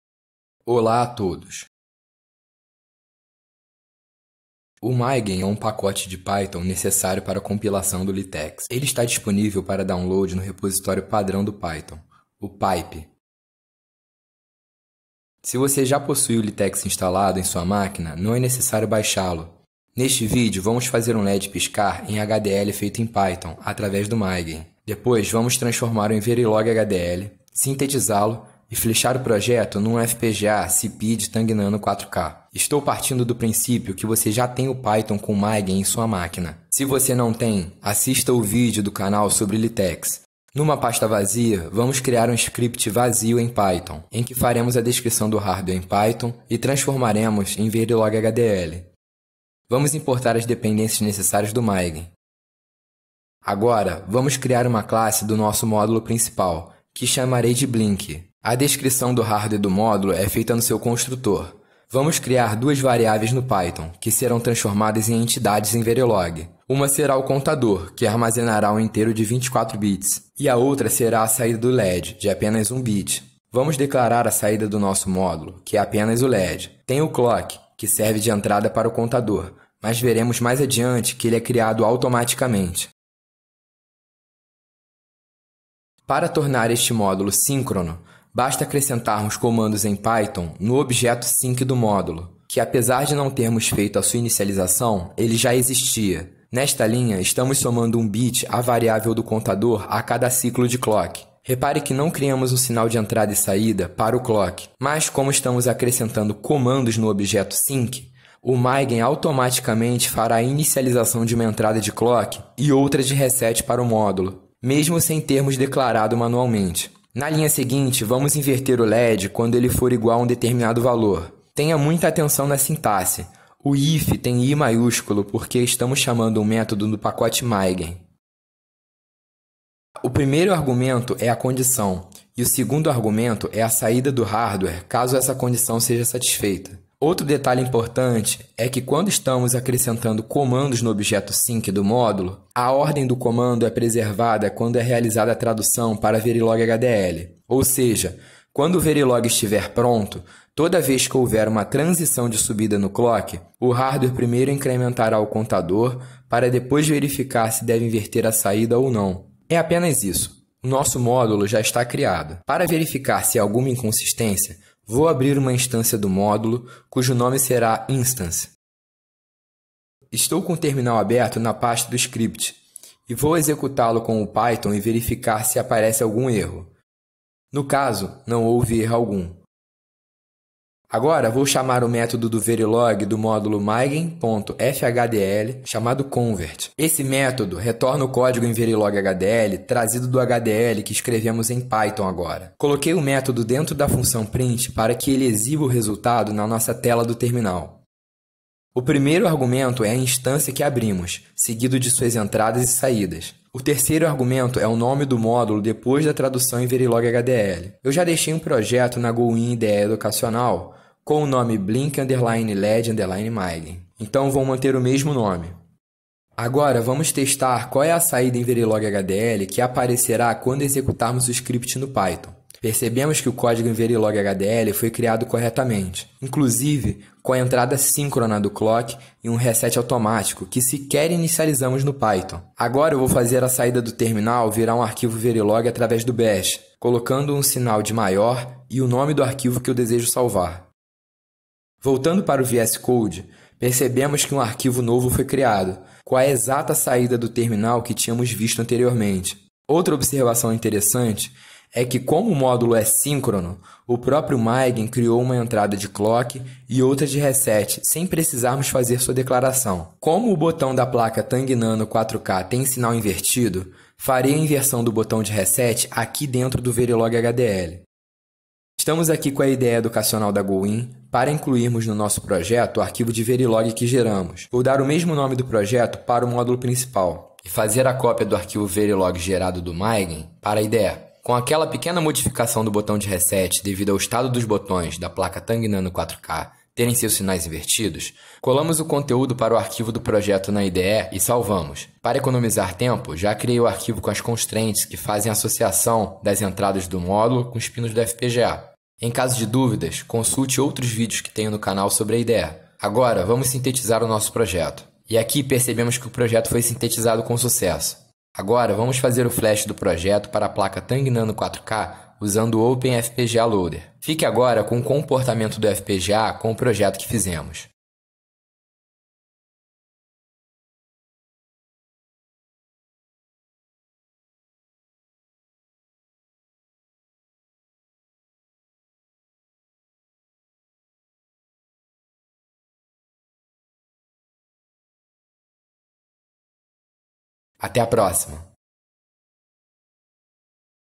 Blink de LED em Python HDL - Tang Nano 4k
FPGA Utilizado: Sipeed Tang Nano 4k

Chapters:
0:00 Introdução
0:41 Implementação do HDL
2:51 Verificação do HDL
2:58 Exportando para Verilog HDL
4:28 Sintetizando o Projeto na IDE
5:08 Flash do FPGA
5:15 Demonstração